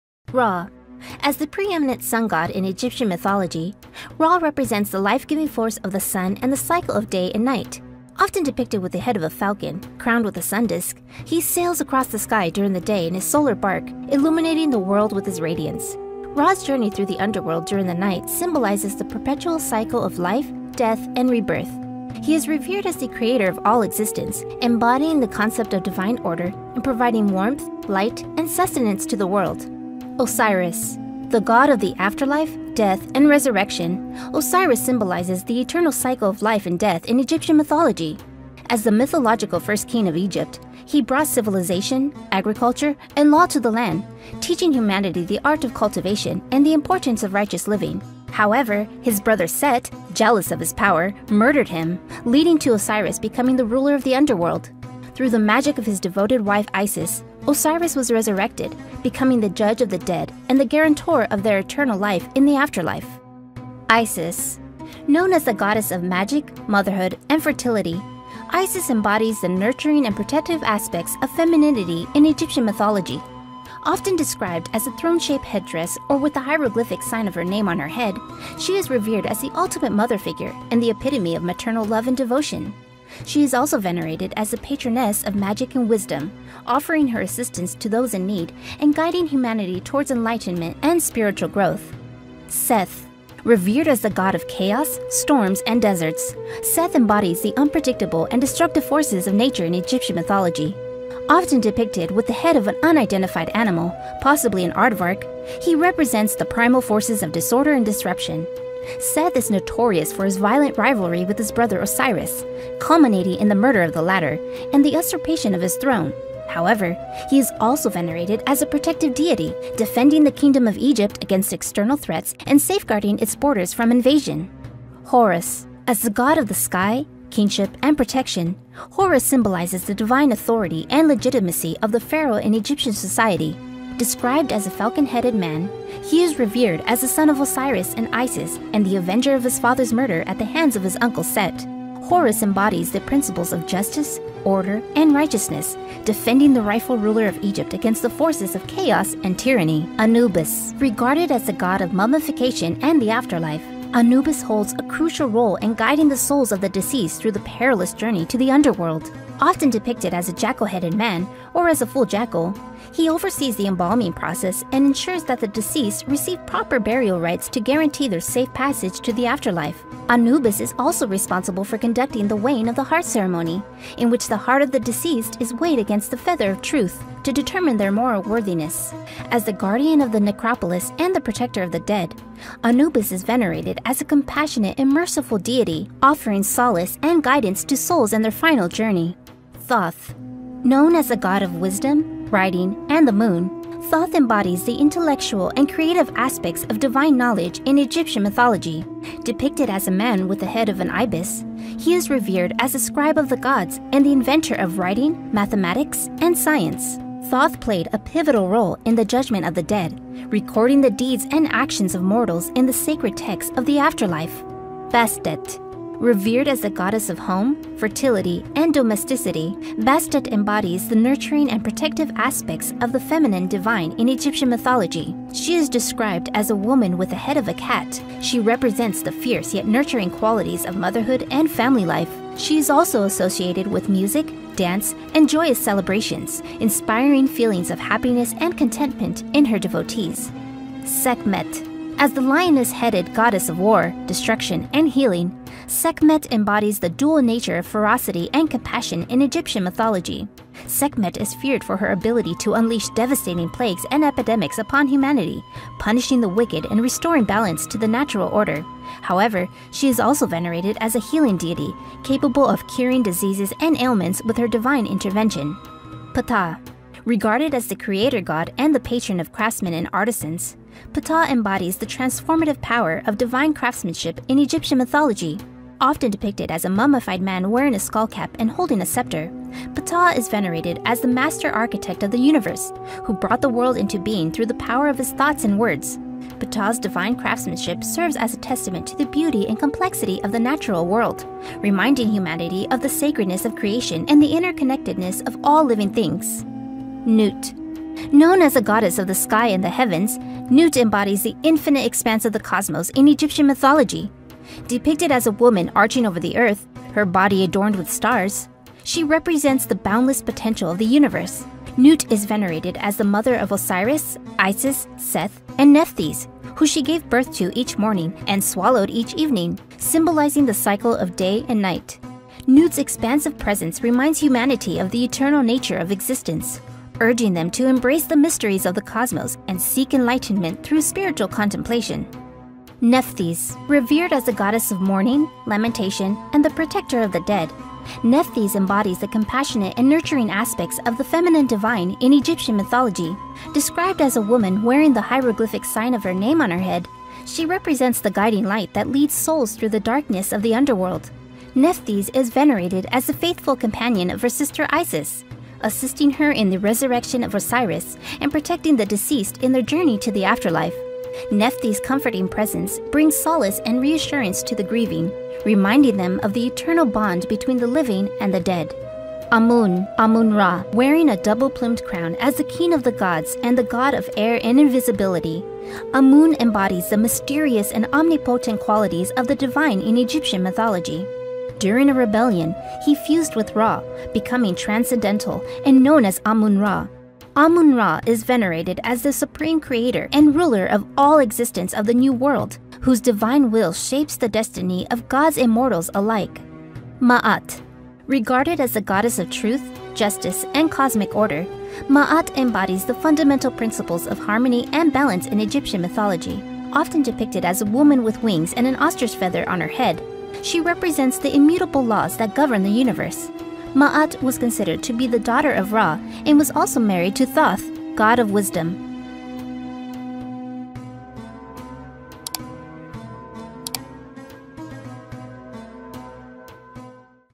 Every Egyptian God Explained in 7 Minutes
In this video I explain every Egyptian God, inspired by The Paint Explainer.

TIMESTAMPS:
0:00 - Ra
0:29 - Osiris
1:03 - Isis
1:31 - Seth
2:04 - Horus
2:29 - Anubis
3:08 - Thoth
3:34 - Bastet
4:00 - Sekhmet
4:25 - Ptah
4:56 - Nut
5:32 - Nephthys
6:08 - Amun (Amun-Ra)
6:36 - Ma'at